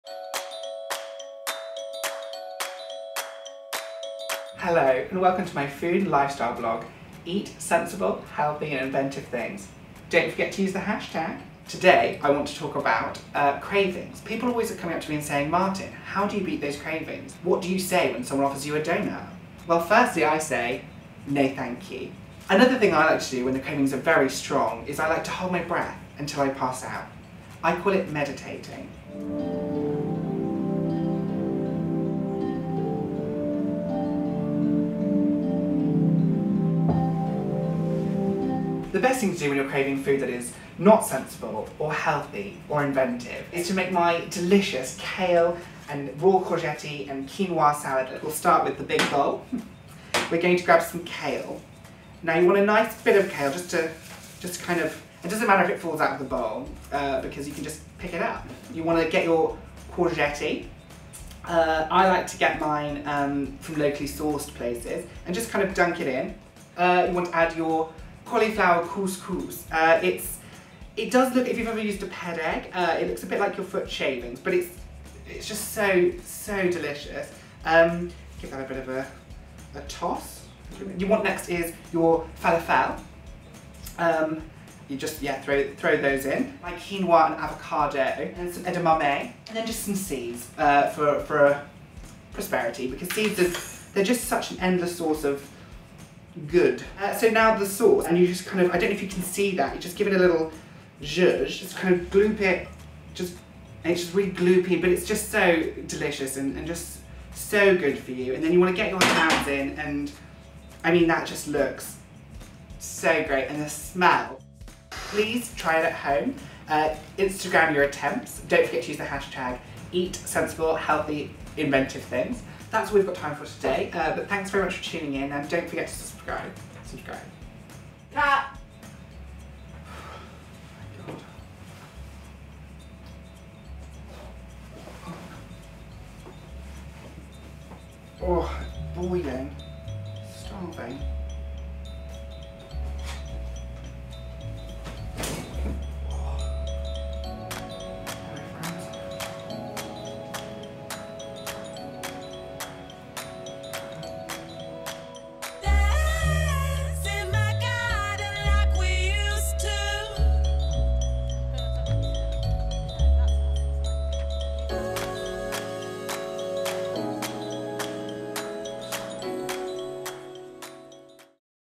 DELICIOUSLY MARTIN
We're all looking to eat a little healthier right? I have a Youtube channel so I'm basically an expert. Here's how to make something so healthy and delicious it will kill you, but it will take longer than doughnuts. You're welcome...